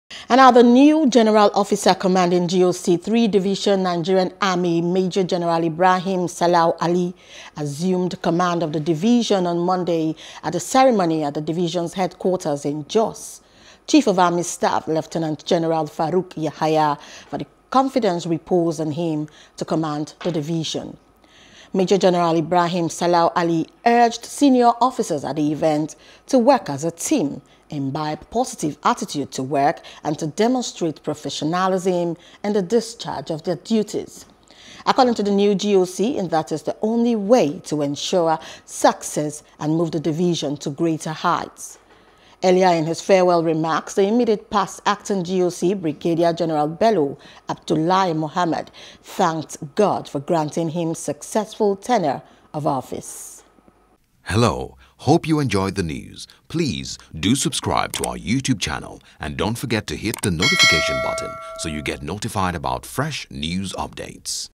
Maj Gen Ibrahim Ali Takes Over As General Officer Commanding 3 Division | NEWS
The new General Officer Commanding (GOC) 3 Division Nigerian Army, Major General Ibrahim Sallau Ali assumed command of the Division on Monday at a ceremony at the Division's Headquarters in Jos.